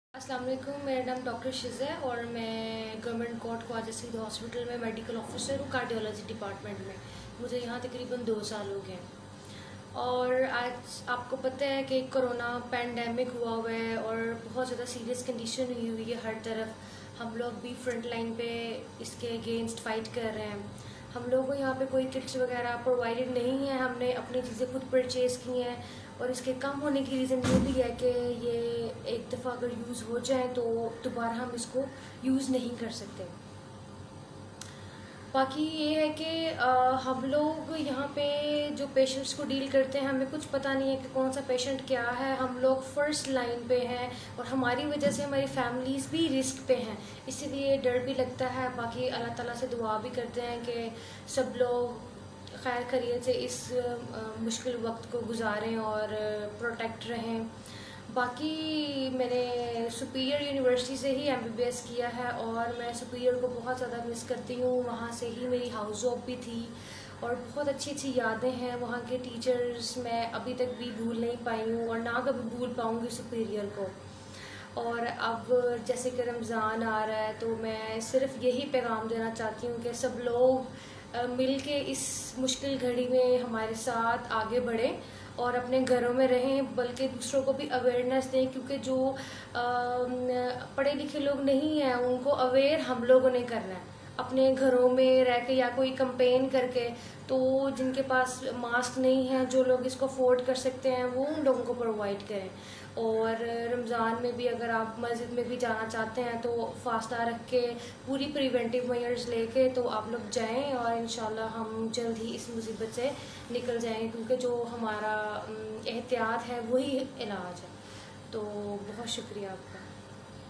Success@Superior-Alumni Stories Dr Sheeza Zubair
Let's hear to our Alumni Dr. Sheeza Zubair (MBBS - Azra Naheed Medical College), expressing her feelings for Superior Family and sharing her routine as a doctor / Front-Line-Protector during prevailing conditions due to this pandemic COVID19.
*If you are also contributing towards society in this pandemic or you want to share your quarantine time with us, then just create a short video and inbox us on our page.